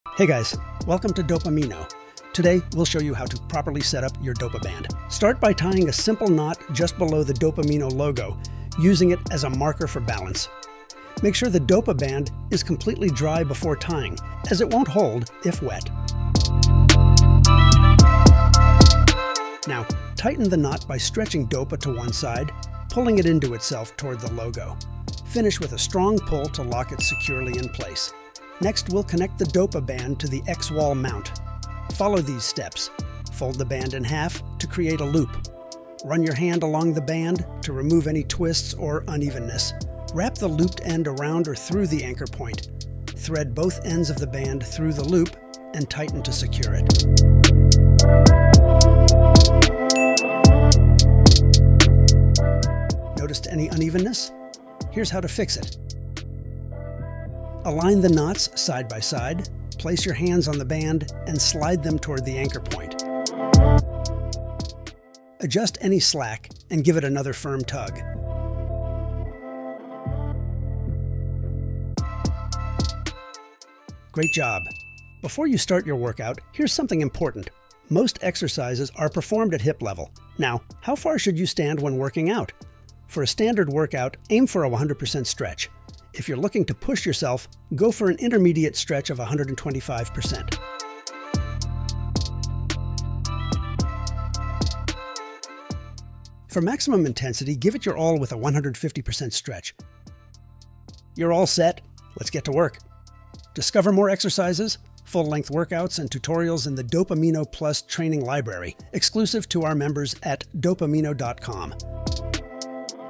New! Fully Explained Dopa Setup Course - Welcome to DopamineO
Setup Your Dopa Resistance Bands: Essential Instructions

Learn how to set up your Dopa Bands for maximum results! This step-by-step guide will help you master the basics and get the most out of your Dopa Band workouts.

👉 How to Make Knots for Grip
Secure a firm grip by learning the best techniques to tie knots with your Dopa Bands.

👉 Attaching Dopa Bands to Anchor Points
Find out how to properly secure your Dopa Bands to different anchor points for stable and effective resistance.

👉 Balancing Your Dopa Bands
Ensure both sides of your Dopa Bands are evenly balanced for smooth resistance.

👉 Adjusting Workout Height and Distance
Position the band at hip level and adjust distance for your workout needs:
100% for Standard, 125% for Intense, and 150% for Max Intensity.

🔥 Ready to Train Anytime, Anywhere?
Check out more workout tips and explore our full range of Dopa Bands

Links you may find helpful:

📦 Don’t have Dopa Bands yet?

Timecodes:
0:00 - intro
0:05 - Making Knots For Holding Grip
0:28 - How to Tie Dopa to an Anchor Point
0:52 - Ensuring Dopa Is Balanced
1:14 - Workout Height and Distance
1:43 - Discover more!